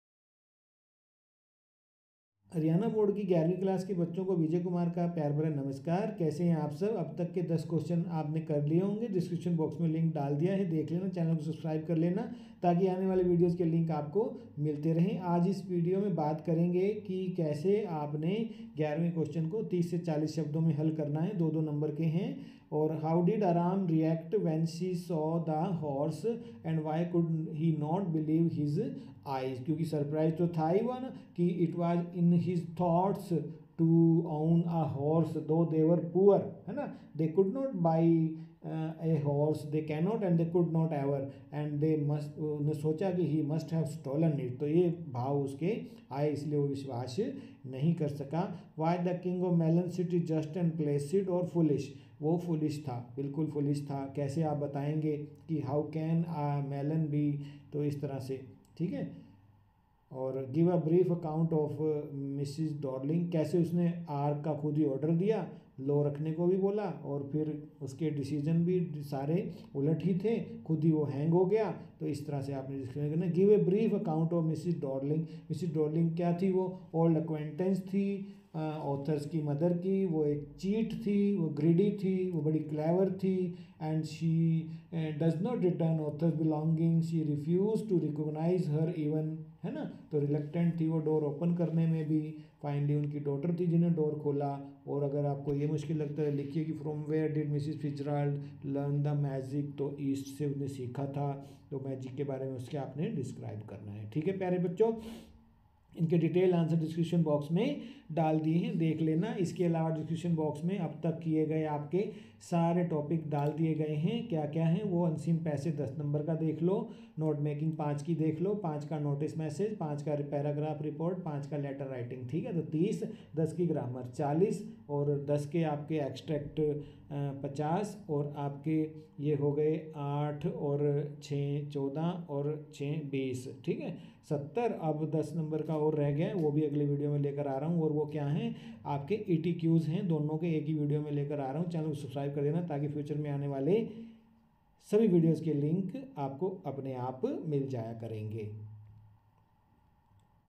important questions snapshots class 11 english hbse cbse by vijay kumar
In this video, we have given important questions from Snapshots book class 11 English Haryana board exams CBSE annual exams 2024 by Vijay Kumar